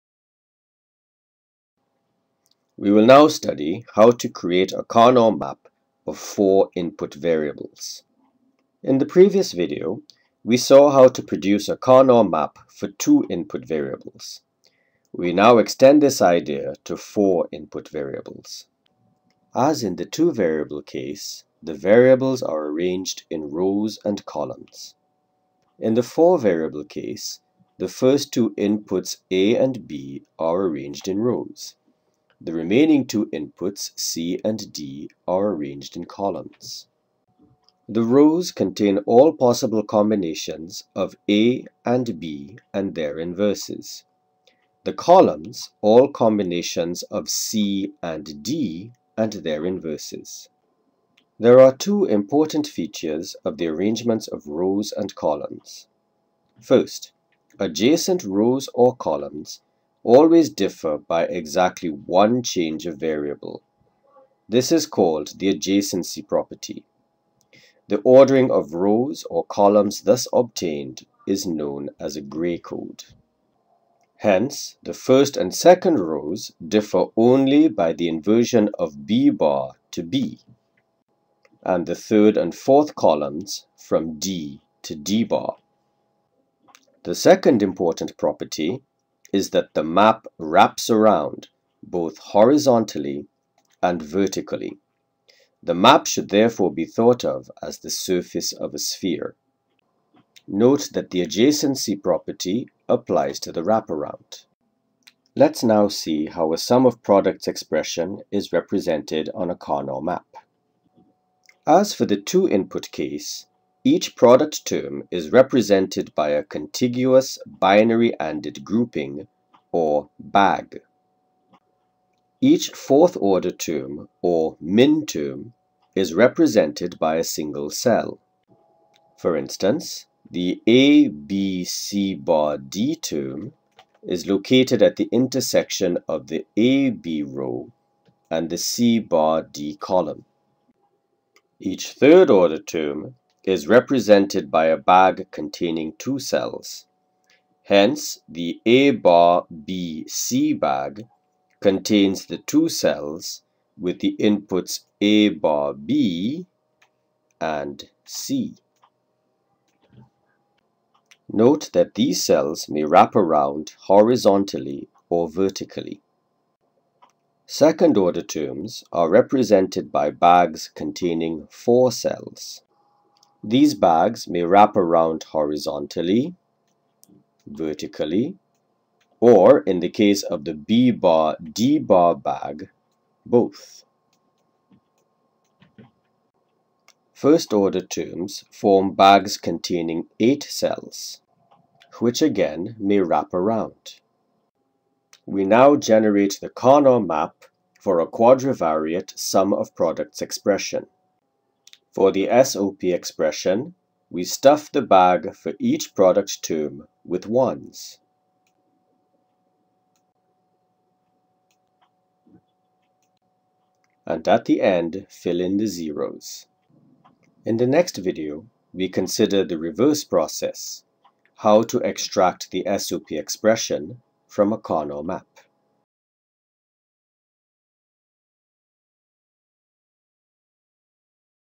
Digital Electronics 10101 - Quadrivariate Karnaugh Map
This video explains the Karnaugh Map for four input variables.
The video is part of an introductory series on Digital Electronics.

Marcel Sankeralli is a Physics Professor at Marianopolis College (Canada).